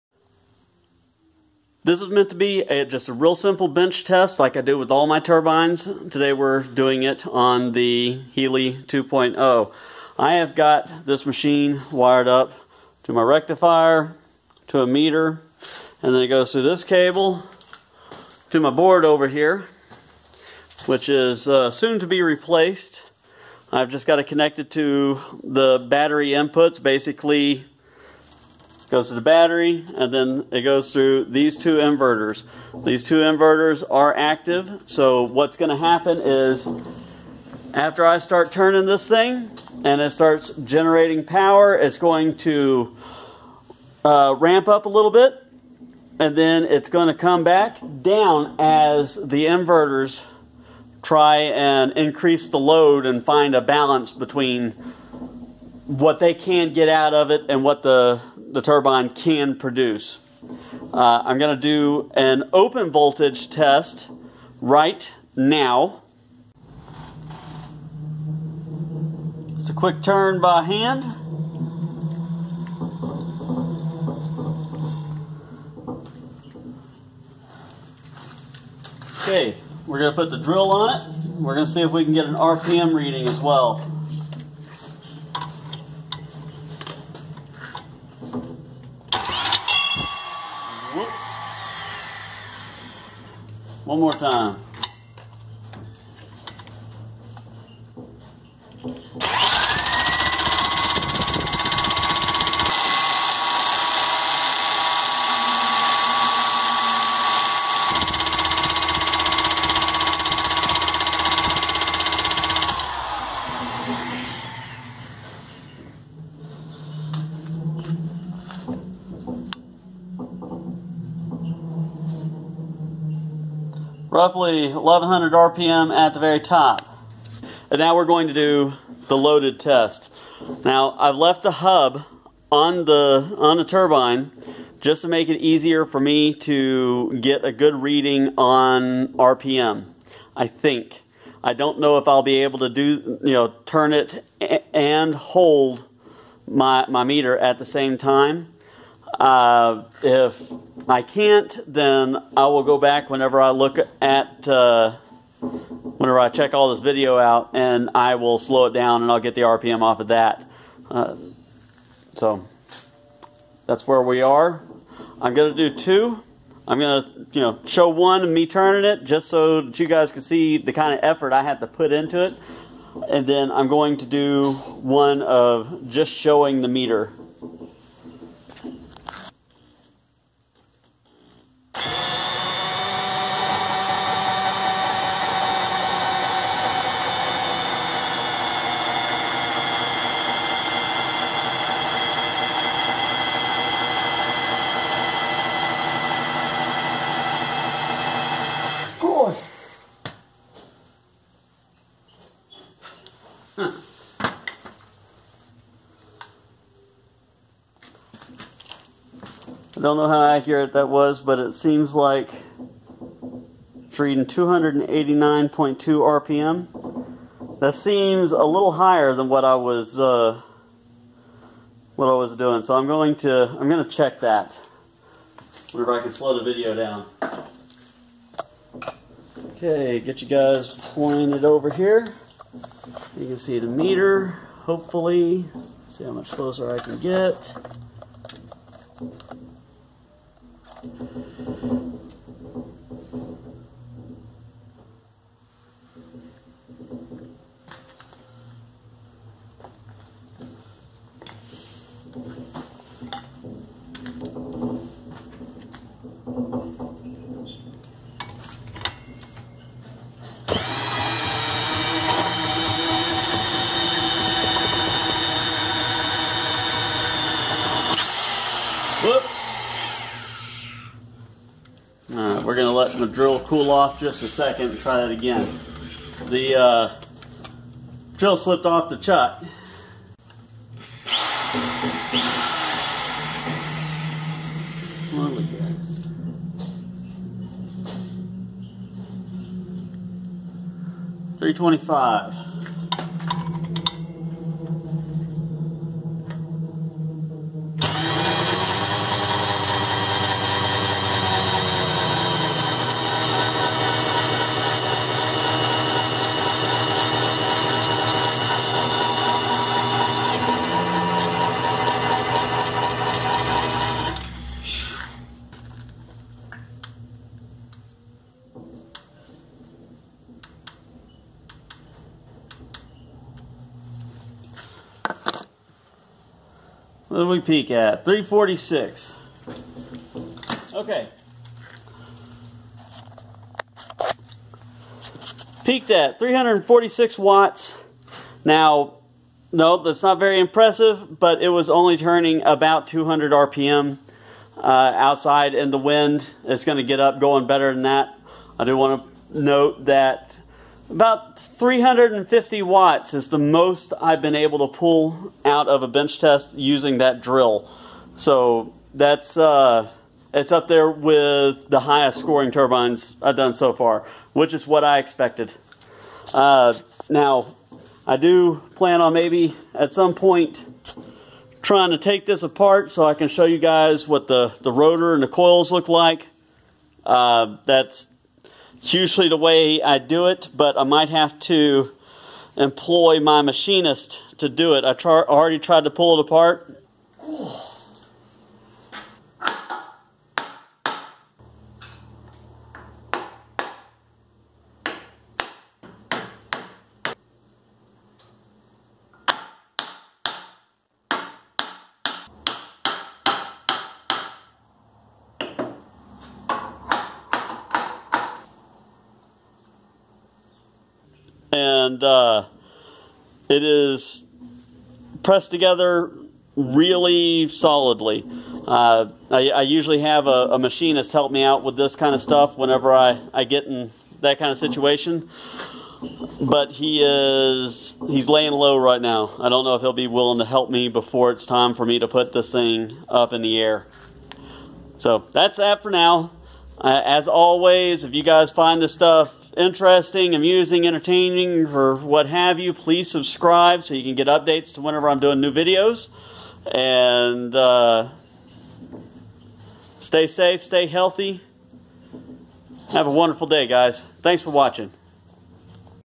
iSta Breeze Heli 2 0 open voltage and initial bench test
Just a touch under 350w burst using the drill, leveling off about 250w after the inverters load the thing up.  Holds 350~370w with the inverters disconnected, might go back and take another shot of that (didn't have the camera running for a battery test :/ ).